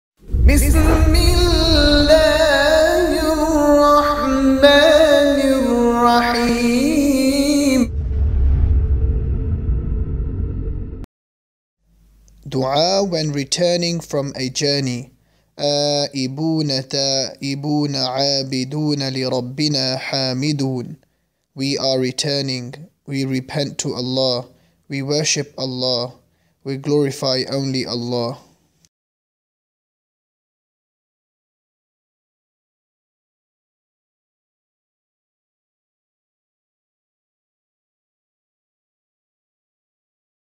Dua For When Returning Form A Journey
« آيِبُونَ تَائِبُونَ عَابِدُونَ لِرَبِّنَا حَامِدُونَ ».
We are returning, we repent to Allah, we worship Allah, we glorify only Allah.